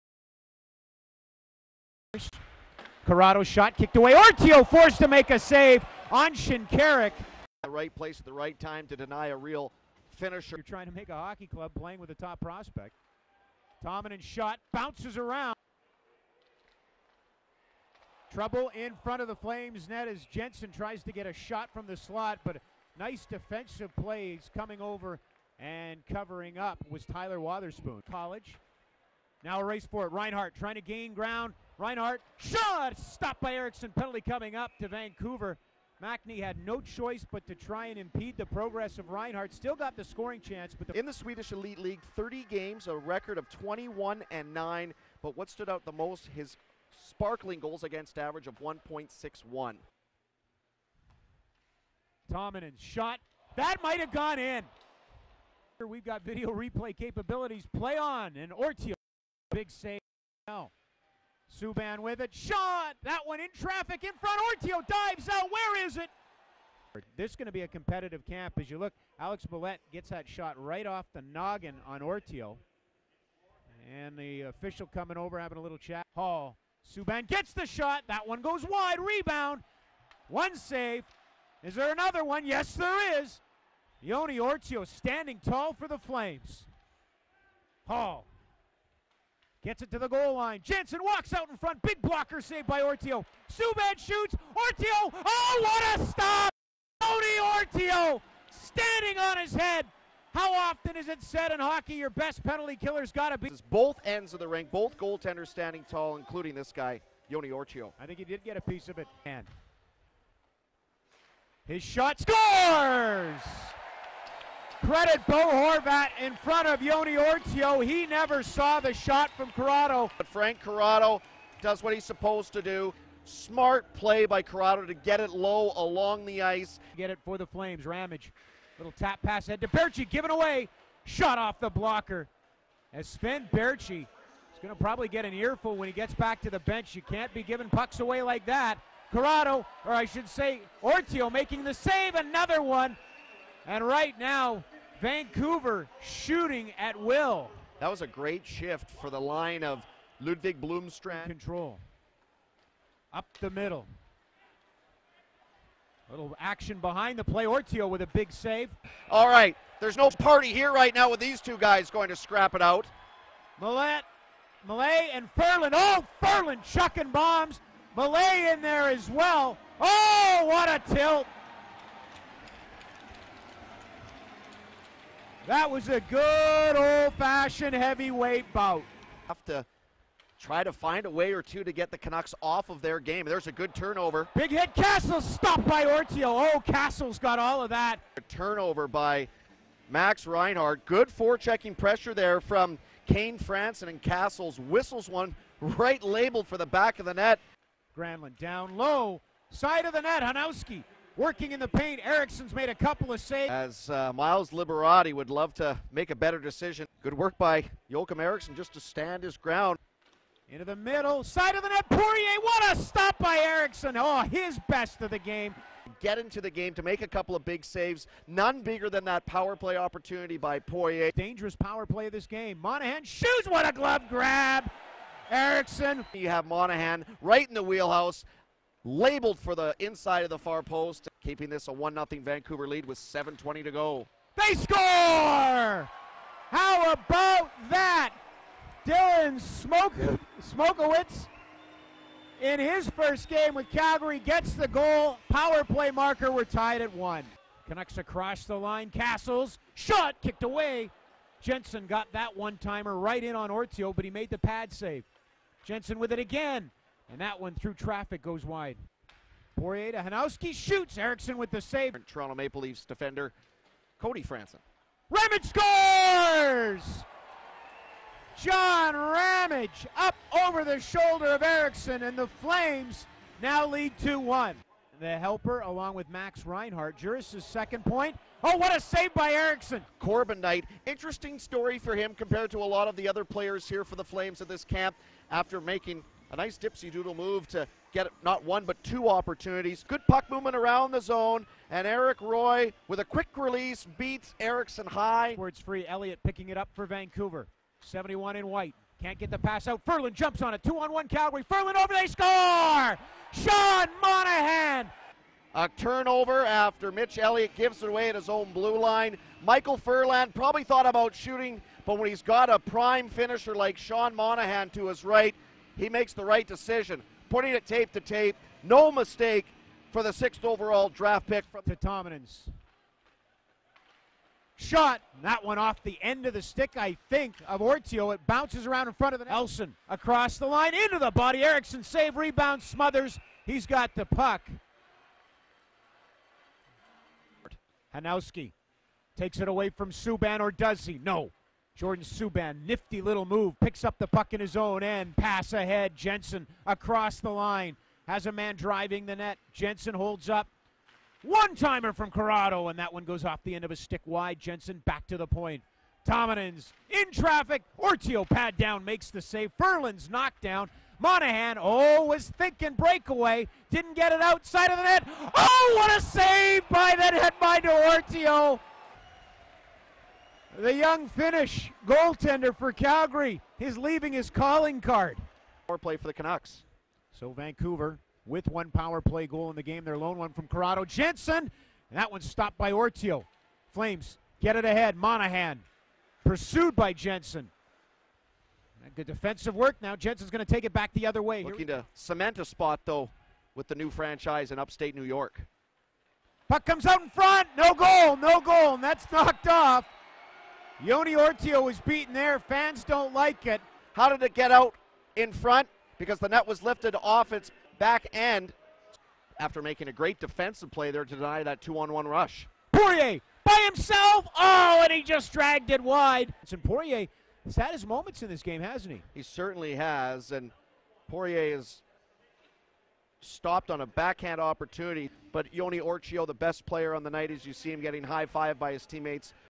2013.09.06 Young Stars Tournament: Canucks Vs Flames
Highlights include all goals, scoring chances, saves and a fight.

Attn Calgary fans, clips are Canuck-focused by design, but the video does feature a few Flames scoring opportunities and the four goals scored by the Flames.